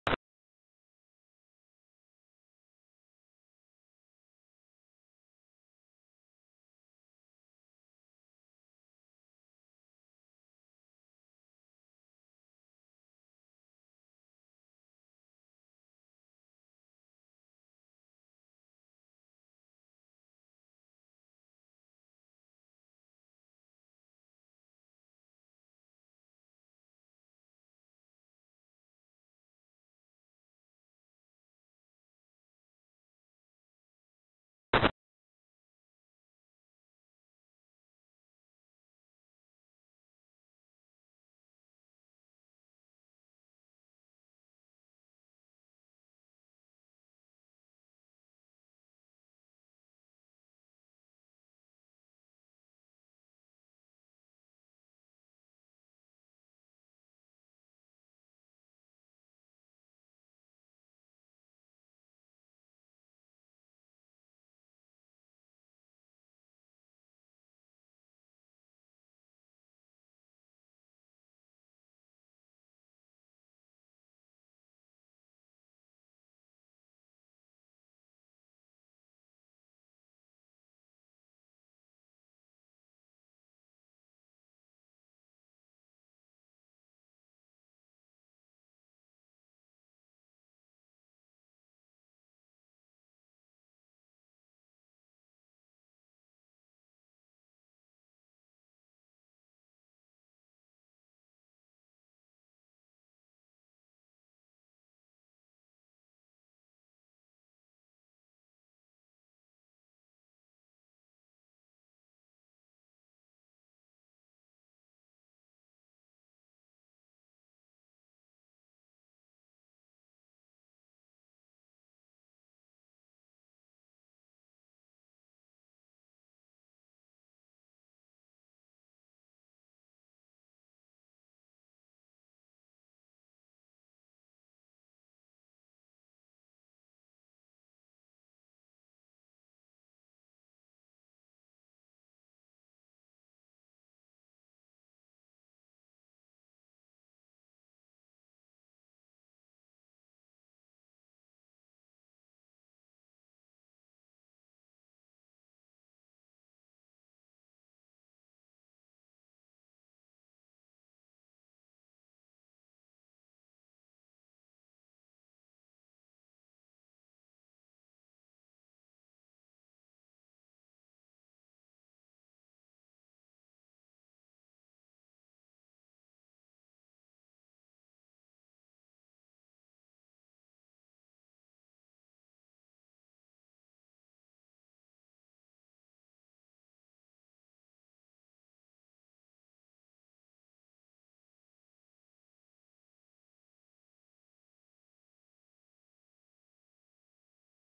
Big Milly - Getting the Monster Mill into the shed
Horne Enterprises moving a Pacific-Lagun FAV-4LA into the shed. The driveway is at a steep angle and the forklift monstered it.